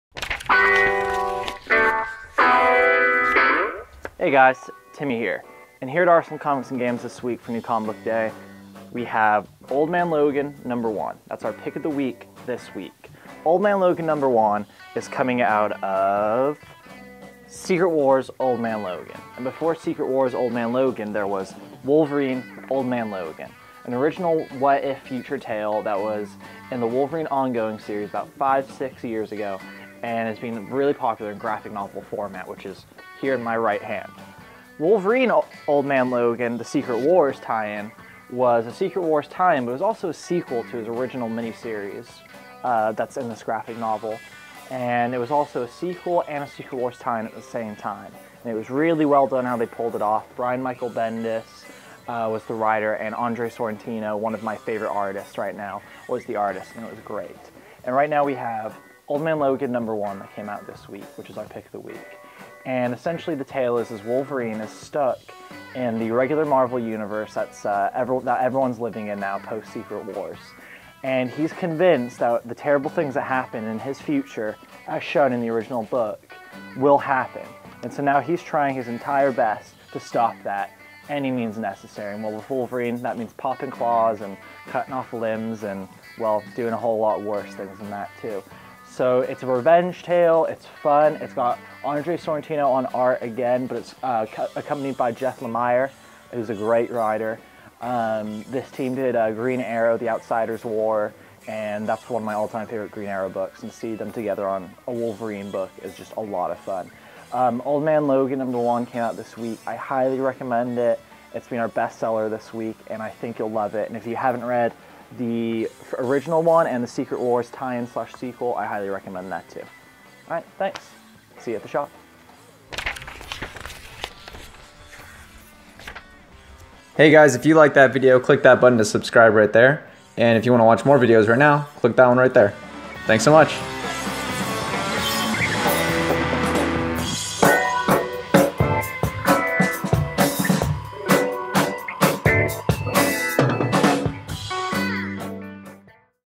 Old Man Logan #1 Review!
Timmy reviews this week's Arsenal Pick, Old Man Logan #1!!